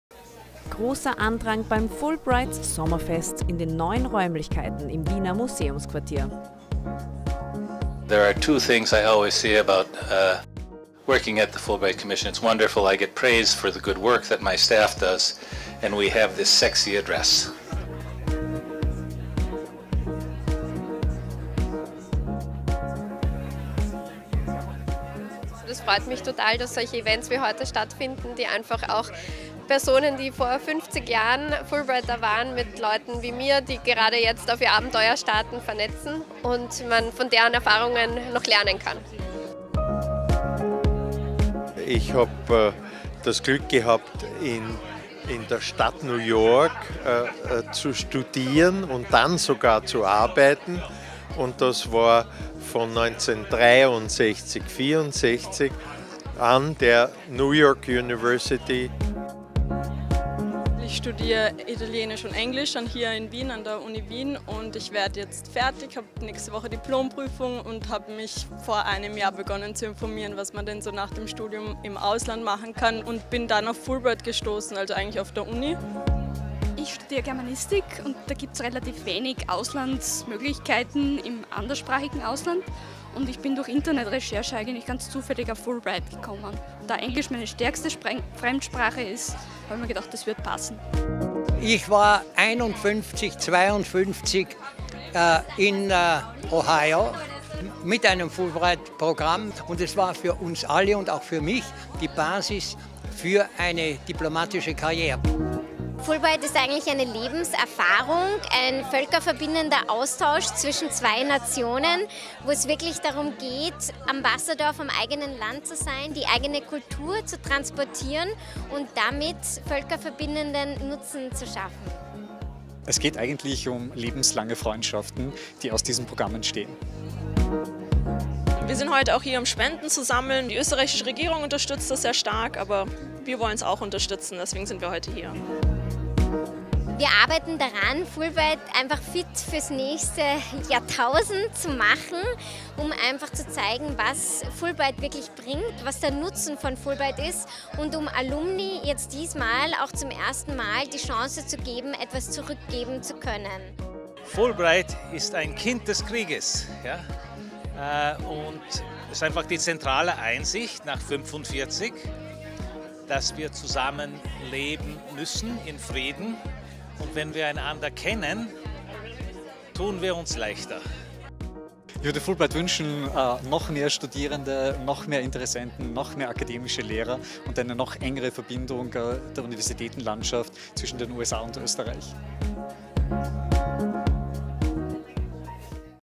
First Annual Fulbright Austria SummerFest
Couldn’t make it to our first-ever Fulbright Austria Summer Fest? Take a peek and see what six decades of Austrian and American alumni had to say about their life-changing Fulbright experience from 1951 to 2018! More than 80 alumni and friends visited the Fulbright Austria office in the MQ – MuseumsQuartier Wien on June 6 and enjoyed the sunshine on our terrace. We also are pleased to report that all donations received for Austrian grants will be matched from an incentive fund of the Austrian Federal Ministry of Education, Science and Research (BMBWF), resulting in more than €13,000 raised this June. Thank you for your incredible support and advocacy for Fulbright Austria! Thanks, TV Salon am Naschmarkt, for the great video.